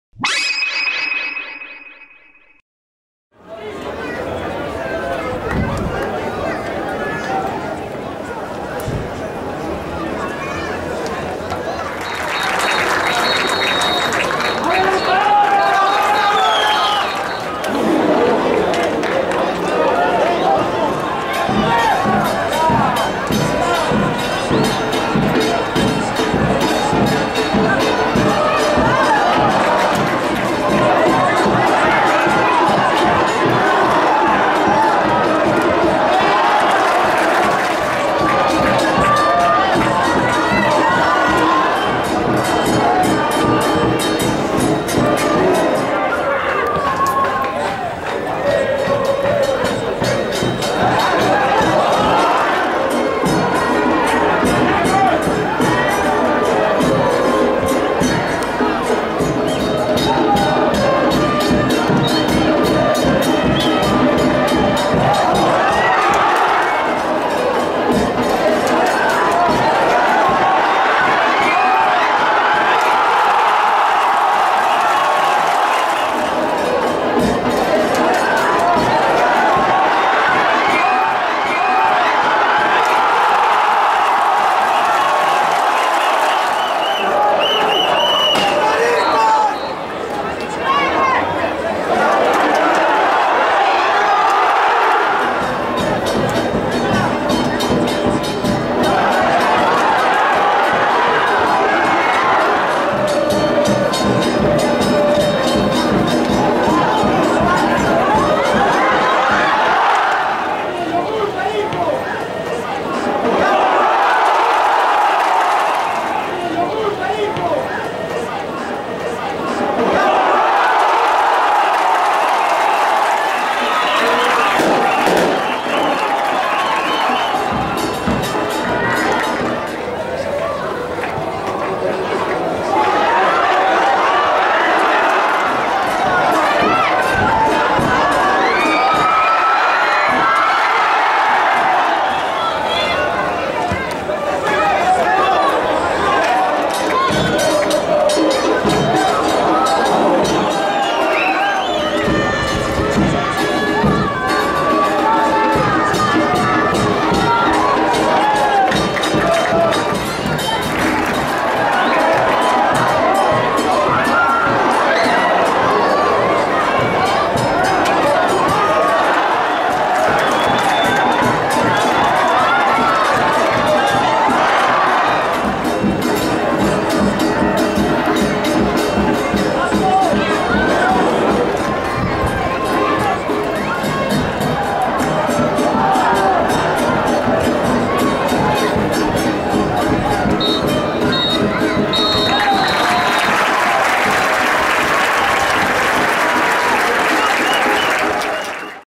AURORA 2 - RAMIRO CASTILLO 0
el equipo del pueblo se impone a  Ramiro Castillo con dos tantos